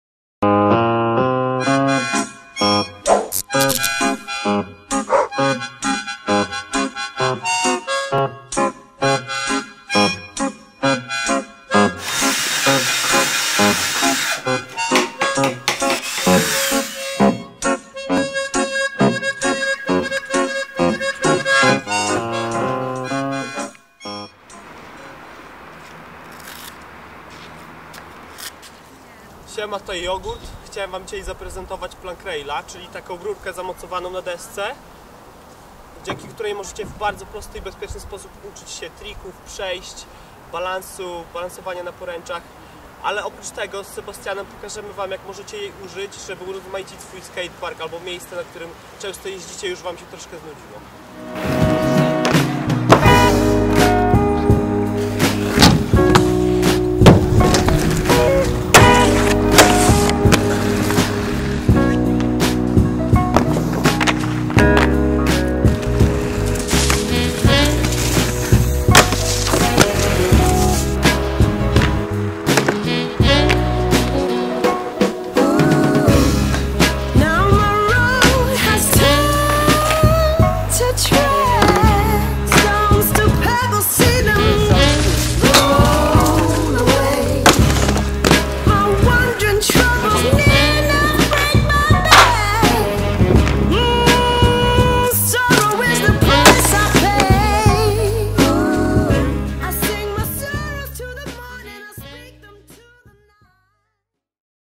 Jazda agresywna na rolkach - nauka grindów na Plank Rail
Jak bezpiecznie trenować jazdę agresywną i grindy w bezpieczny sposób? Najlepiej zrobić sobie Plank Rail ;)